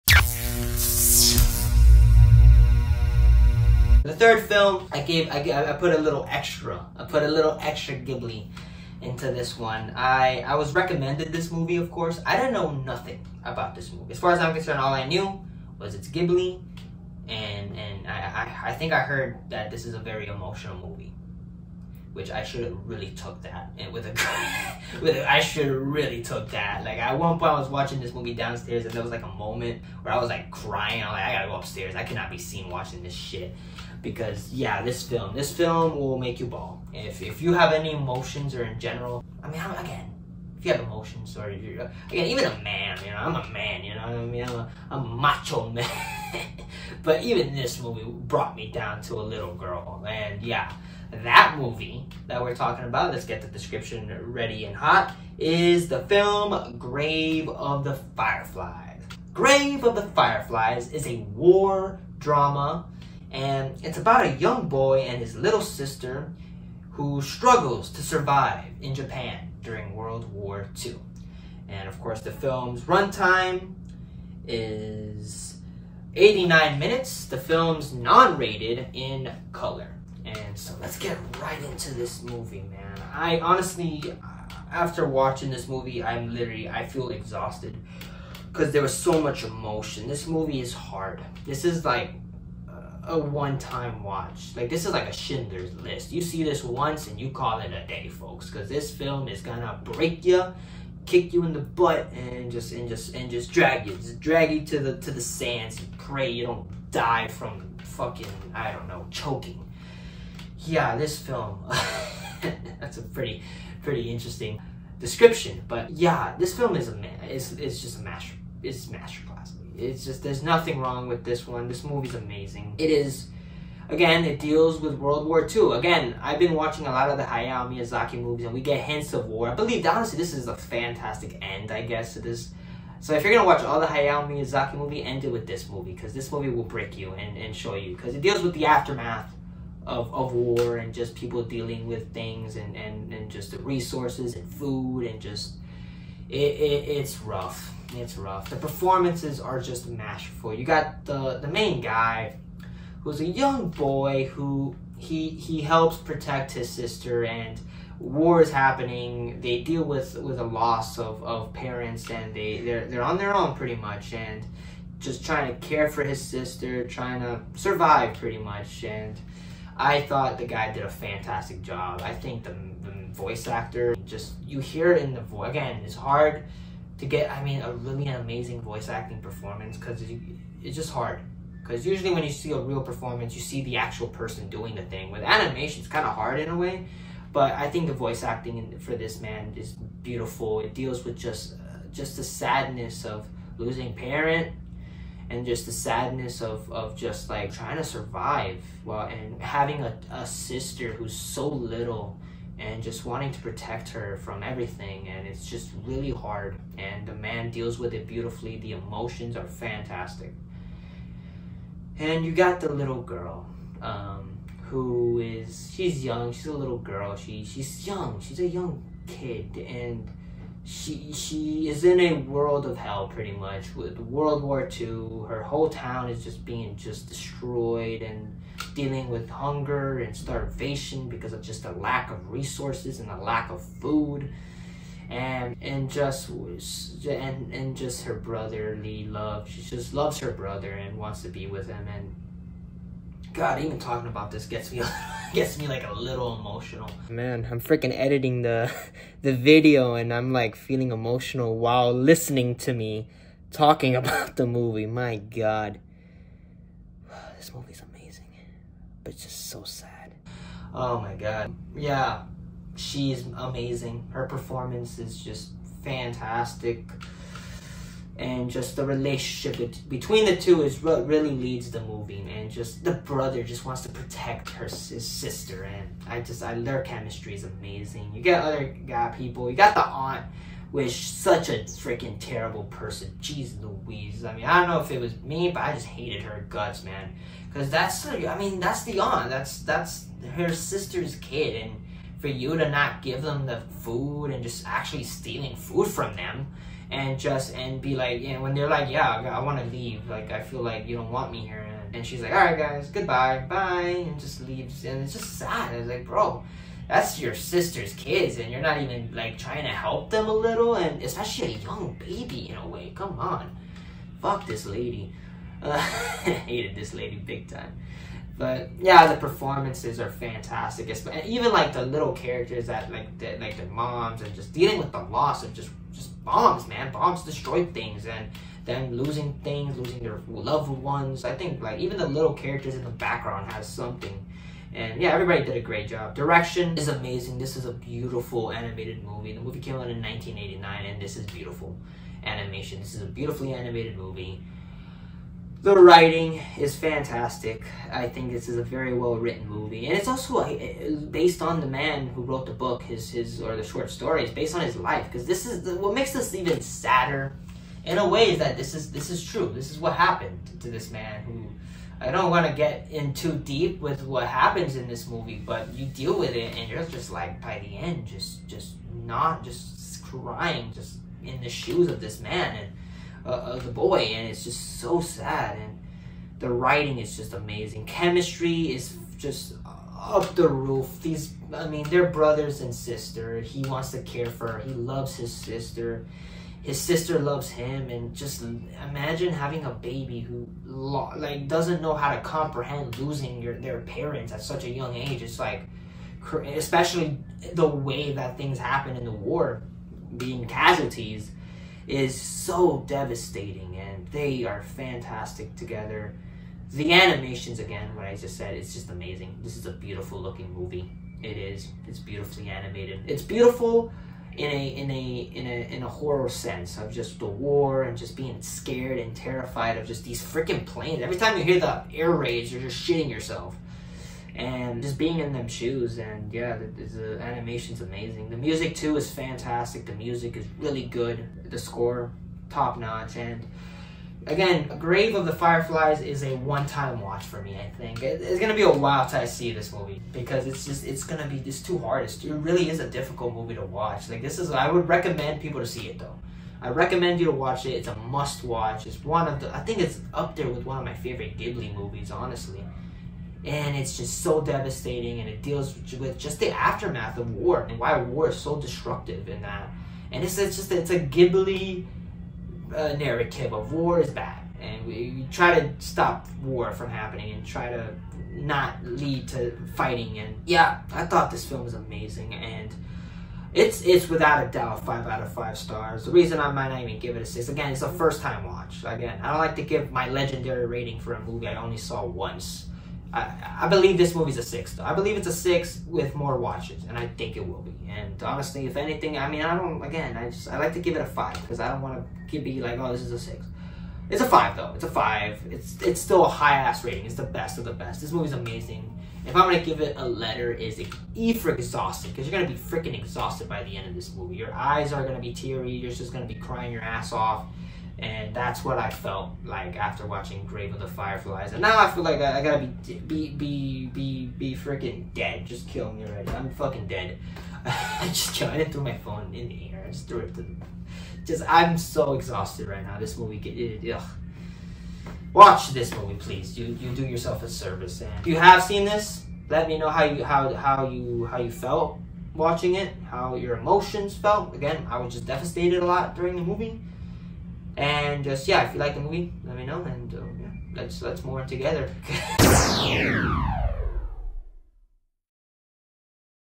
Grave of the Fireflies - Josue Suarez Movie Review
This movie had me absolutely exhausted by the end, and if you know then you know.